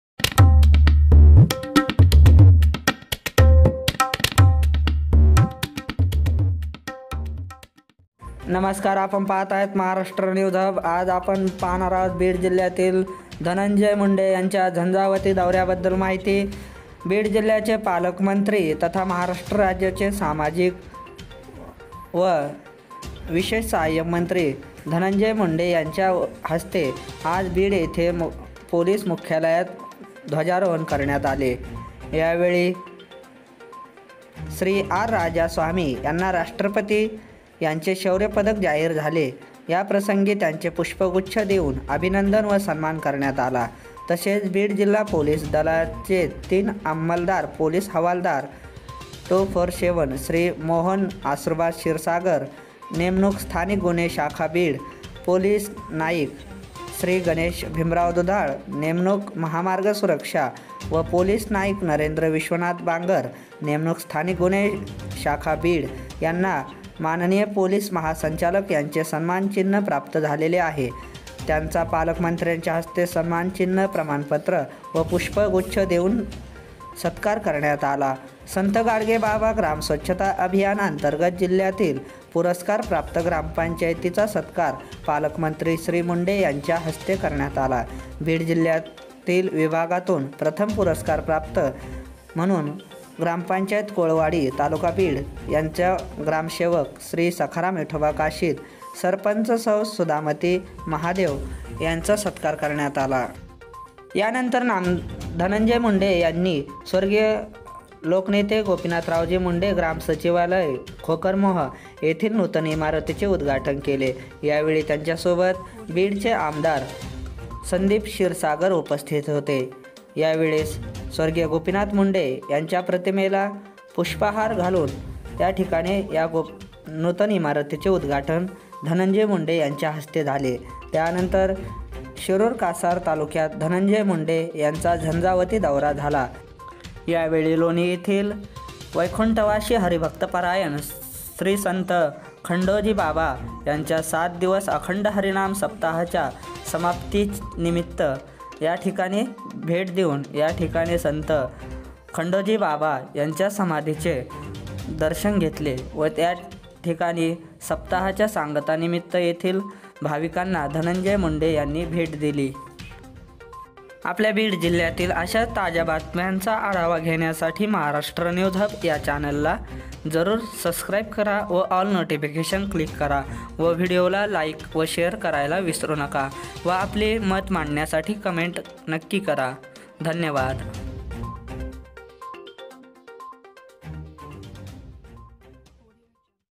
Dhananjay munde यांचा प्रजासत्ताक दिनानिमित्त बीड जिल्हा दौरा
Dhananjay munde